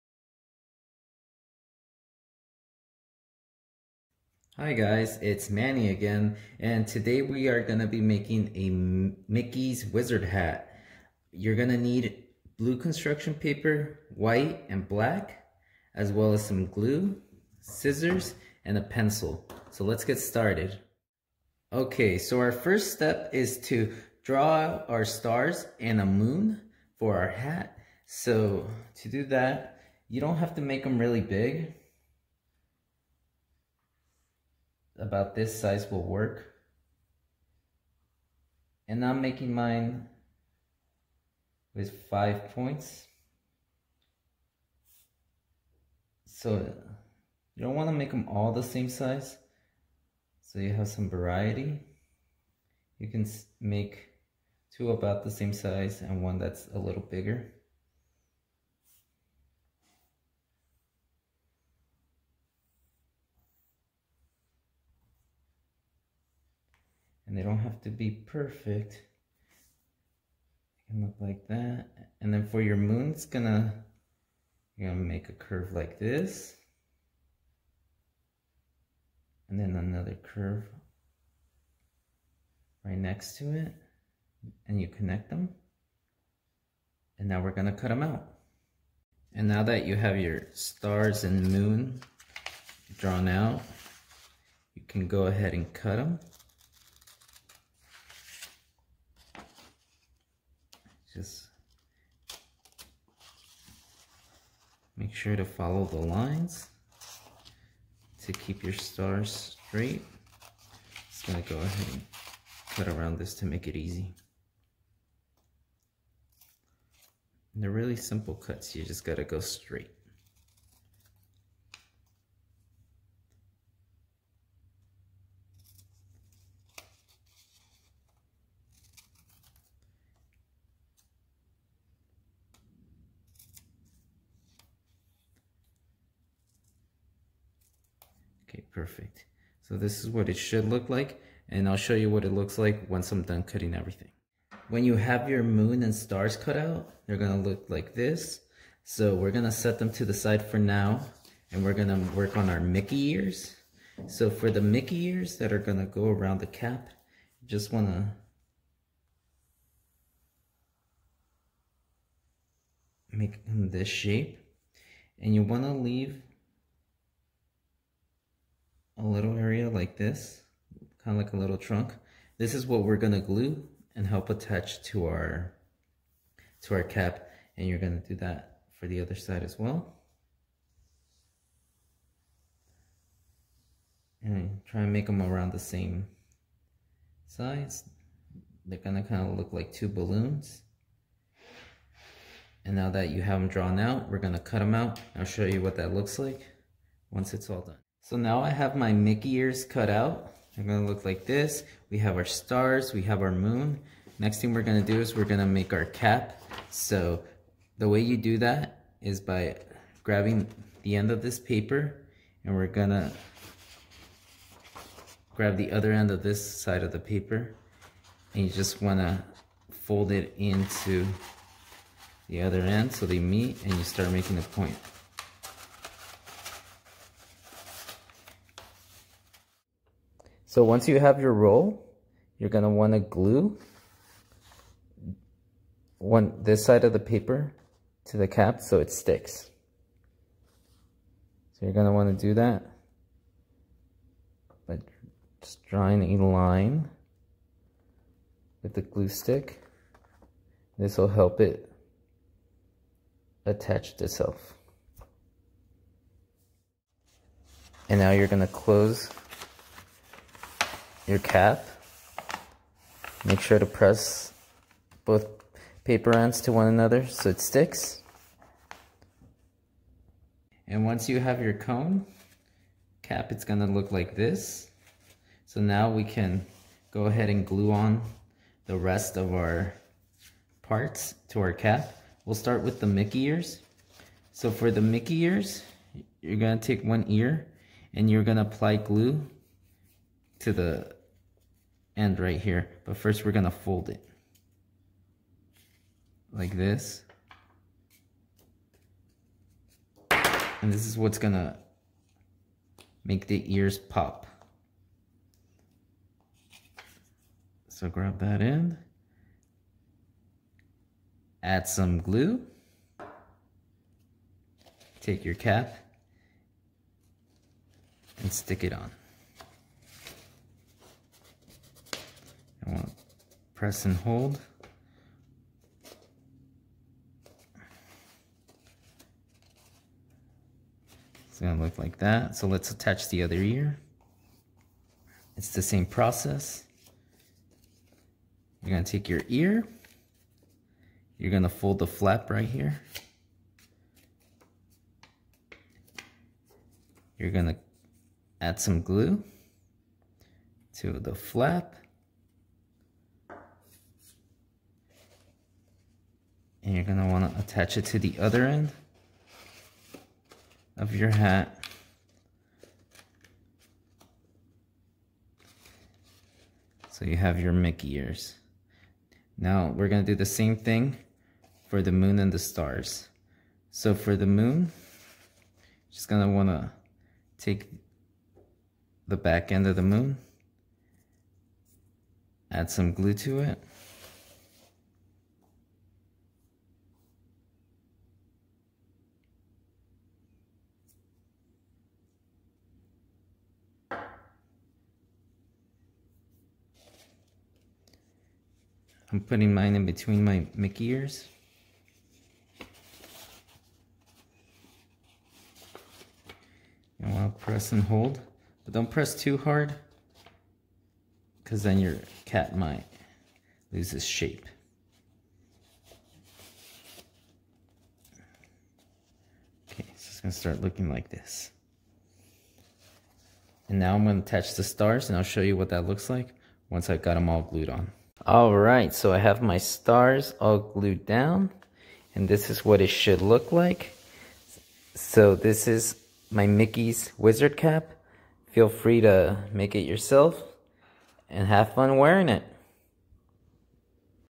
How To Make a Wizard Hat
Hi its Manny. In this video I'll be showing you how to make a wizards hat.